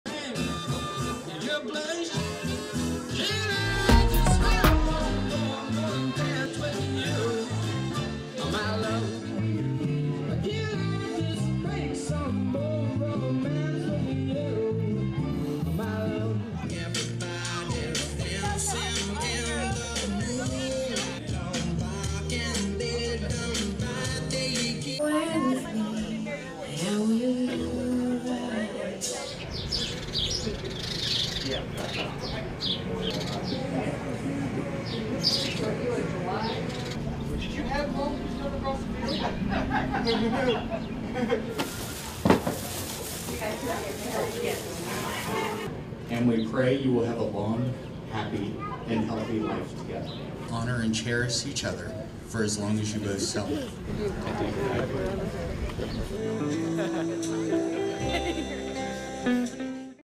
Couples marry during total solar eclipse
More than 20 couples were married during a total solar eclipse at a park in Trenton.

Trenton mayor presided over the brief ceremony that took place minutes before totality.

More than a hundred people came to the park to witness the ceremony and watch the total solar eclipse.